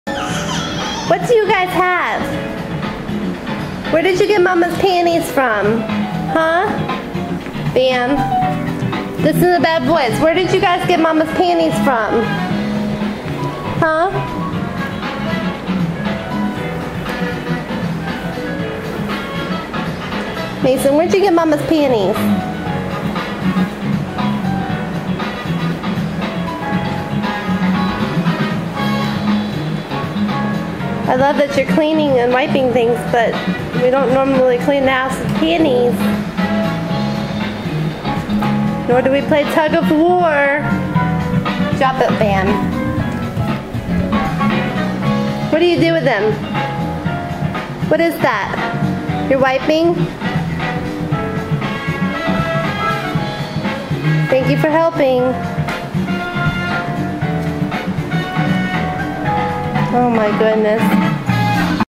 Panty Thieves
Mason got into the *CLEAN* Laundry (lol) and grabbed my panties to clean and play tug of war with bam bam.